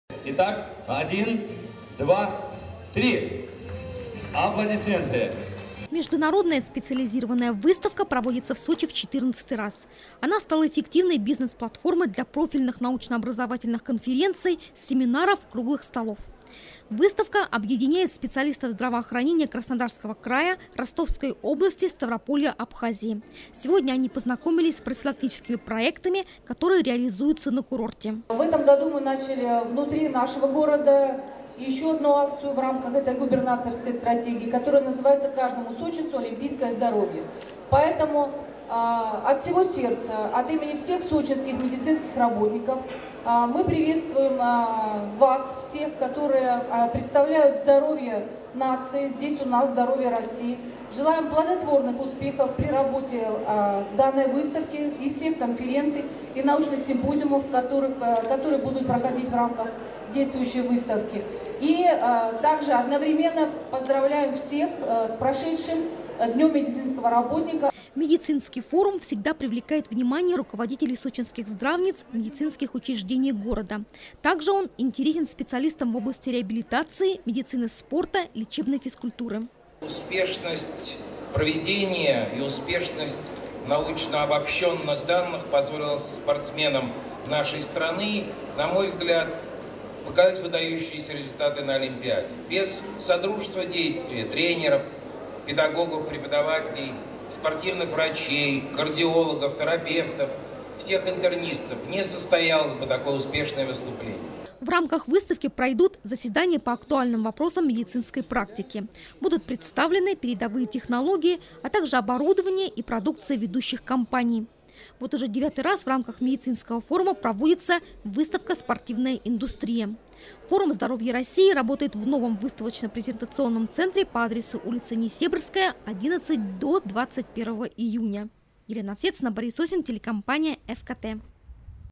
В Сочи открылся форум "Здоровье России" Новости 24 Эфкате Сочи
«Медицина сегодня и завтра». Под таким названием в выставочном центре у Морпорта открылась специализированная выставка медицинской техники, технологий и лекарственных средств для здравоохранения Её организатор — выставочная компания «Сочи — Экспо». Выставка проводится в рамках научно-образовательного форума «Здоровье России. Сочи 2014». Также в выставочном комплексе начала свою работу выставка «Спортивная индустрия».

Международная специализированная выставка проводится в Сочи в 14 раз. Она стала эффективной бизнес-платформой для профильных научно-образовательных конференций, семинаров, круглых столов. Выставка объединяет специалистов здравоохранения Краснодарского края, Ростовской области, Ставрополья, Абхазии. Сегодня они познакомились с профилактическими проектами, которые реализуются на курорте.

Сочи. Новости.
Телеканал Эфкате.